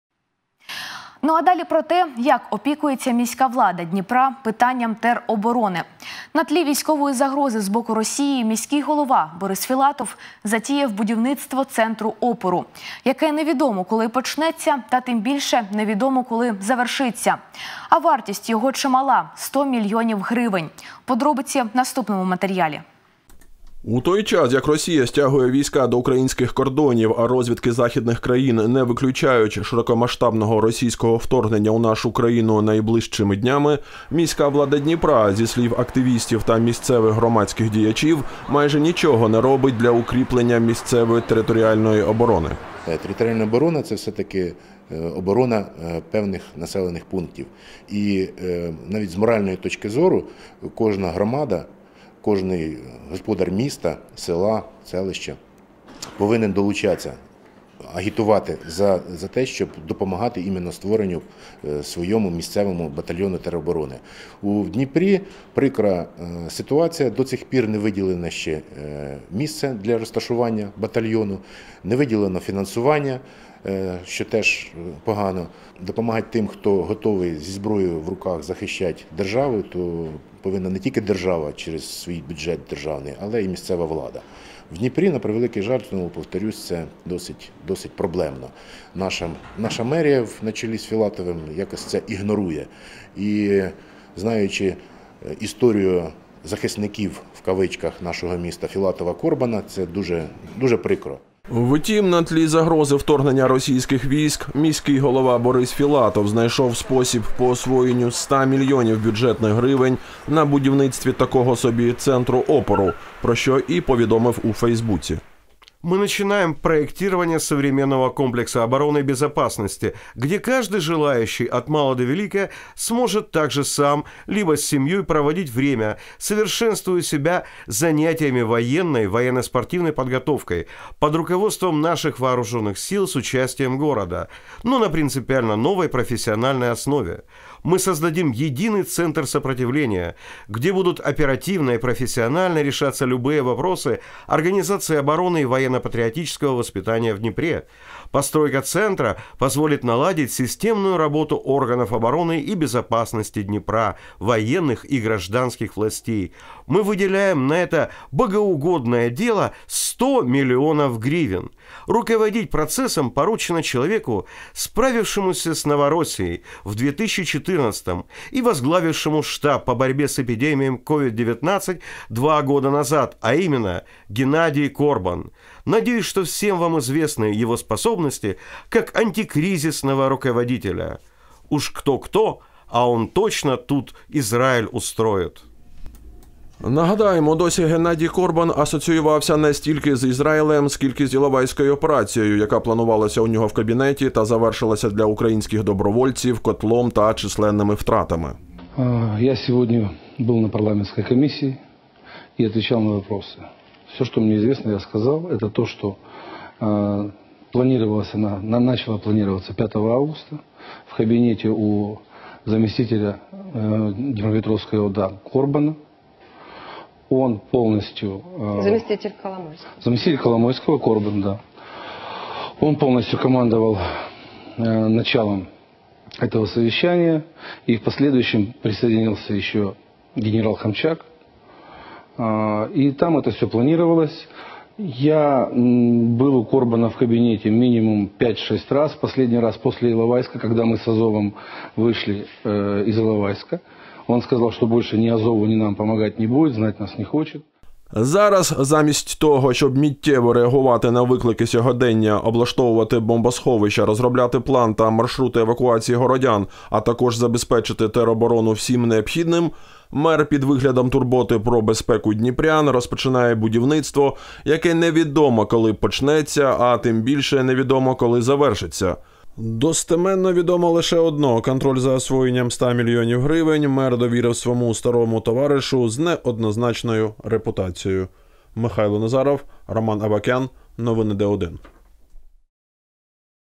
На тлі військової загрози з боку Росії Філатов затіяв за 100 мільйонів будівництво Центру опору
Зараз замість того, щоб миттєво реагувати на виклики сьогодення, облаштовувати бомбосховища, розробляти план та маршрути евакуації городян, а також забезпечити тероборону всім необхідним, мер під виглядом турботи про безпеку дніпрян розпочинає будівництво, яке невідомо, коли почнеться, а, тим більше, невідомо коли завершиться. Достеменно відомо лише одно – контроль за освоєнням ста мільйонів гривень мер довірив своєму старому товаришУ з неоднозначною репутацією.